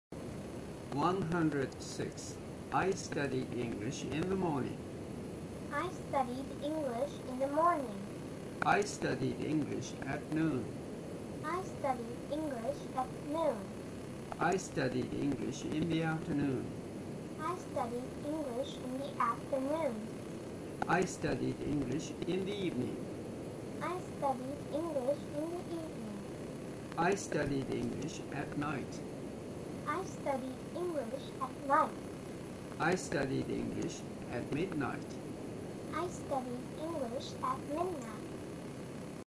英検パスポート ５級例文編１０６
106.
I studied English in the morning.
I studied English at noon.
I studied English in the afternoon.
I studied English in the evening.
I studied English at night.
I studied English at midnight.